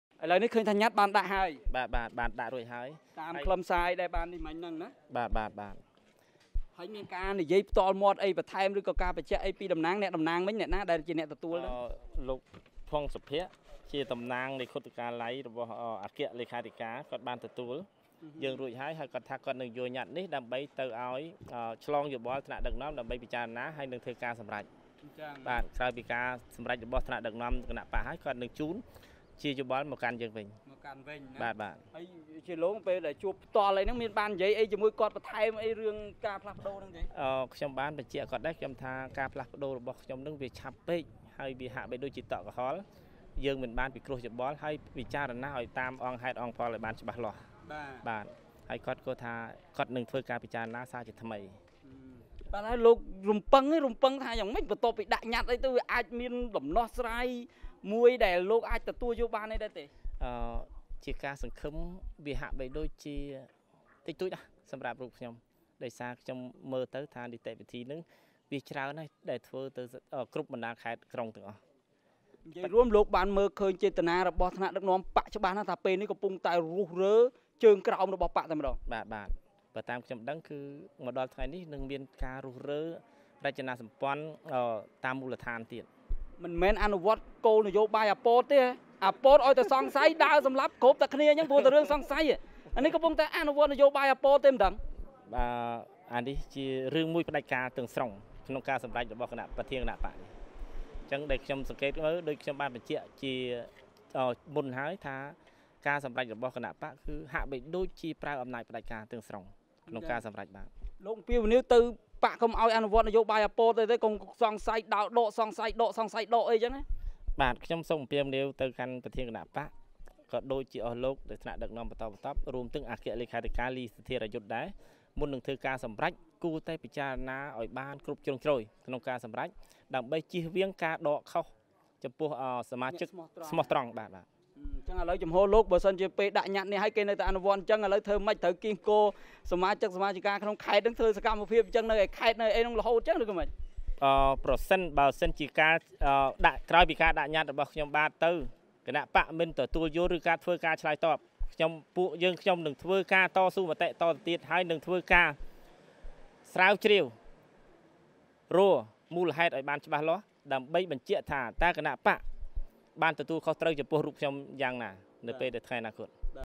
ប្រធានស្តីទីបក្សភ្លើងទៀនខេត្តពោធិ៍សាត់ ផ្តាំមេដឹកនាំបក្សភ្លើងទៀន កុំអនុវត្តន៍...
ប្រធានស្តីទីបក្សភ្លើងទៀនខេត្តពោធិ៍សាត់ ផ្តាំមេដឹកនាំបក្សភ្លើងទៀន កុំអនុវត្តន៍នយោបាយ ប៉ុល ពត គ្រាន់តែសង្ស័យសម្លាប់ គ្រាន់តែសង្ស័យដកតួនាទី